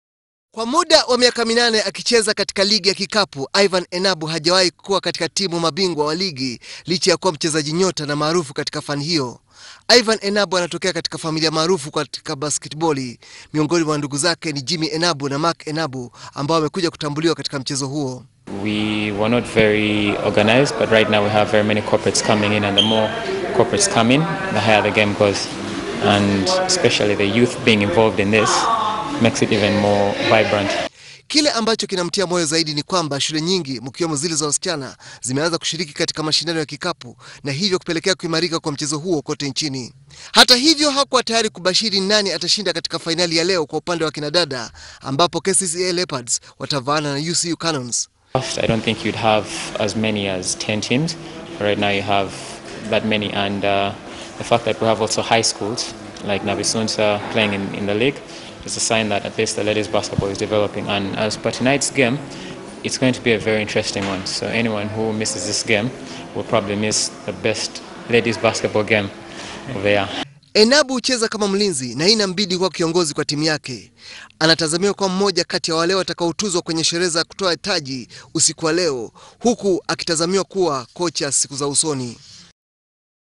BASKETBALL UPDATES IN Tariffa
Mchezaji machachari wa kulipwa katika mchezo wa kikapu ivan enabu amasisitiza kuwa mungu ndiye aliyewawezesha kutwaa taji la ligi ya kitaifa.
Enabu hajawahi kuchezea timu iliyoshinda taji hilo na hivyo ana anatazama ushindi huu kuwa Baraka kubwa kwake.
Hii ni baada ya warriors kushinda mechi ya sita katika jumla na mechi saba za fainali hapo jana.
Waliwapiku falcons klwa jumla ya michezo mine kwa miwili.